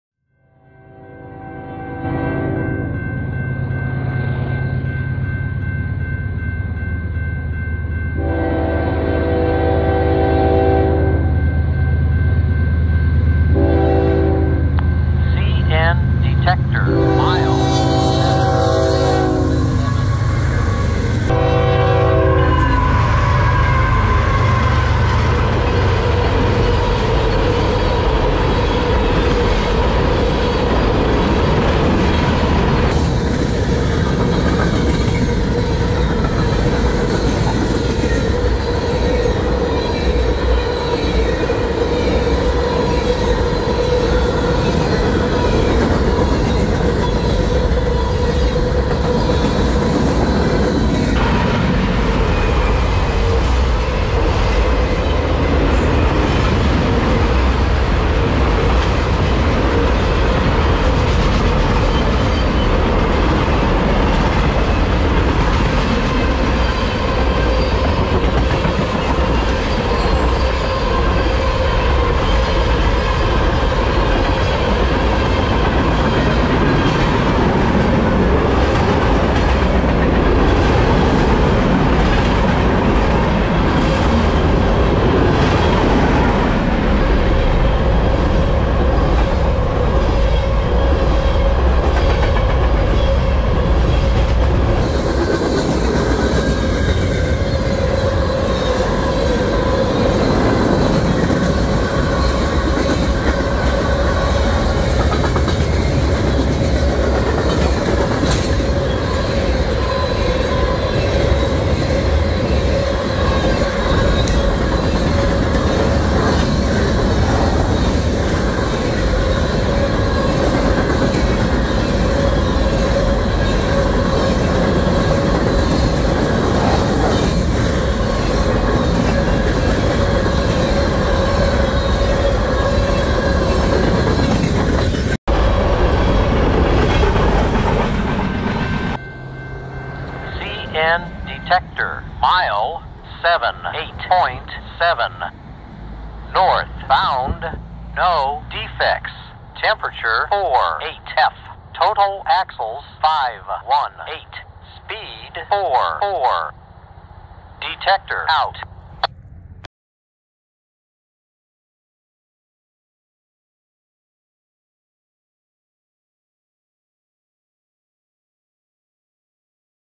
EX-CITIRAIL on a mixed freight PLUS TRACKSIDE DETECTOR AUDIO INCLUDED on the CN
Canadian National 2982 leads a Northbound manifest tarin with CN 2754 trailing on the Canadian national Waukesha Subdivision  in southeast Wisconsin on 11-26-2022. Note the trackside defect detector at the end of the video.